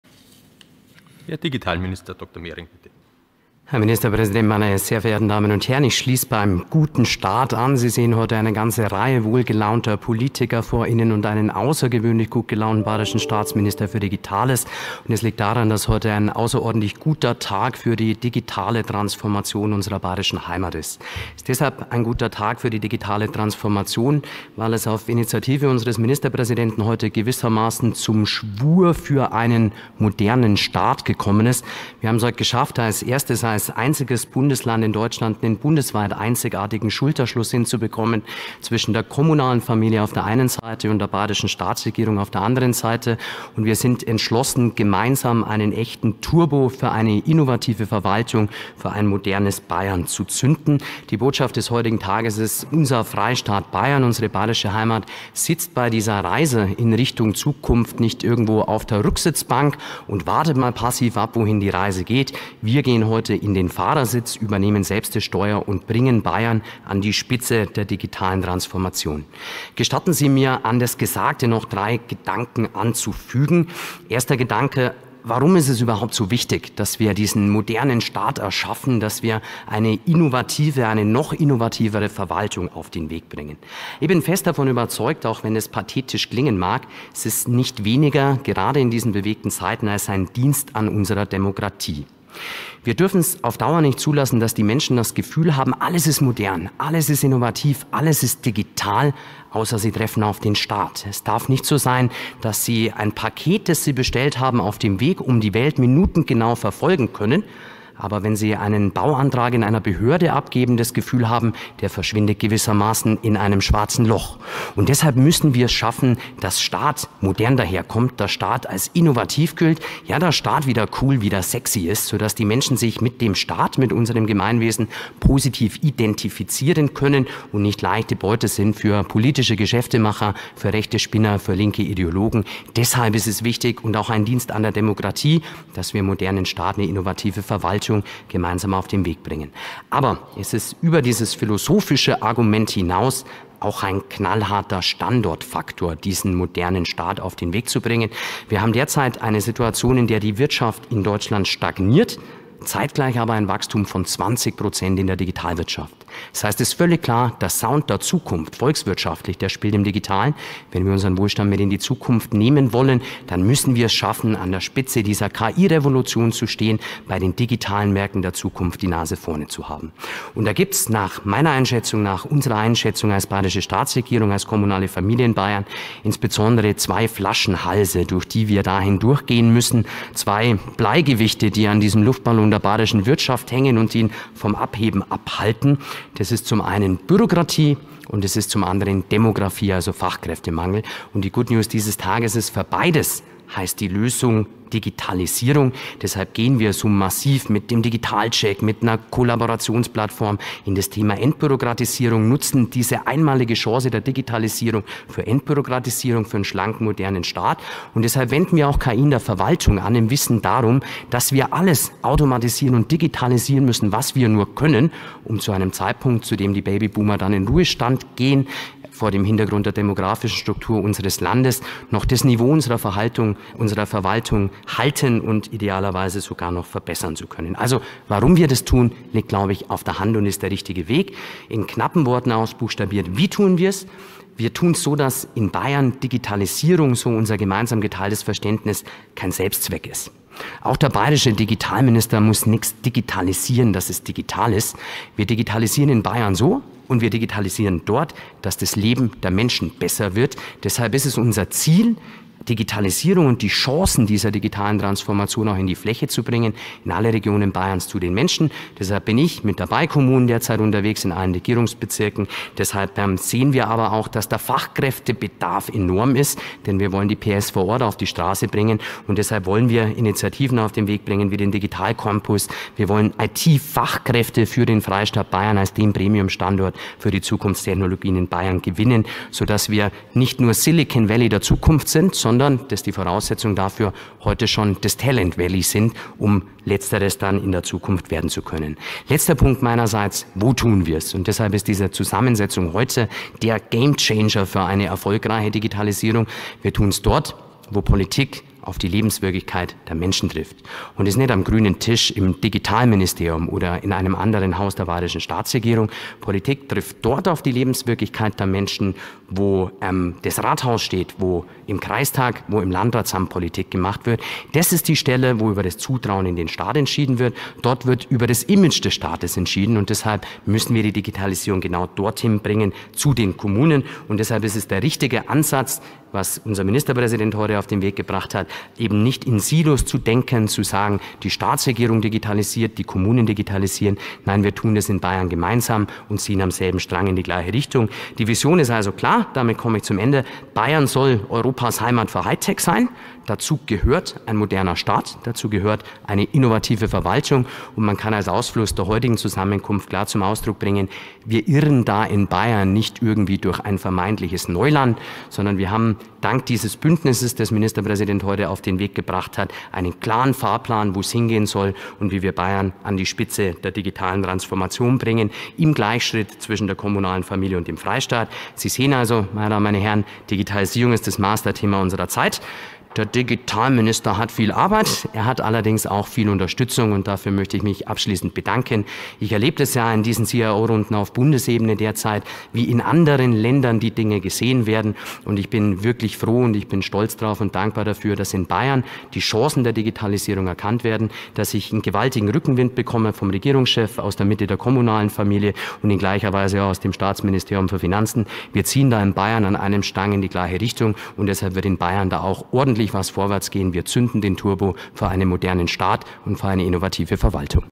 Staatsminister Dr. Fabian Mehring, MdL/ 14.03.2024 - Pressekonferenz "Digitales Bayern"
Der Staatsminister für Digitales und Landtagsabgeordnete der FREIEWÄHLER im Bayerischen Landtag, Dr. Fabian Mehring, in der Pressekonferenz zu "Digitales Bayern" am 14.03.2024.